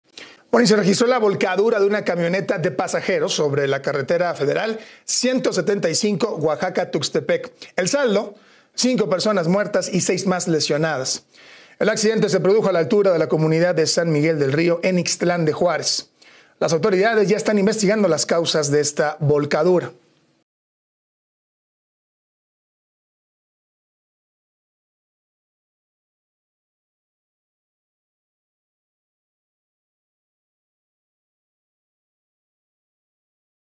Volcadura de camión deja 5 muertos y 6 heridos en Ixtlán de Juárez, Oaxaca - Las Noticias
En Ixtlán de Juárez, Oaxaca, la volcadura de un camión de pasajeros dejó al menos 5 personas muertas y 6 heridas; servicios de emergencia rescataron a las personas.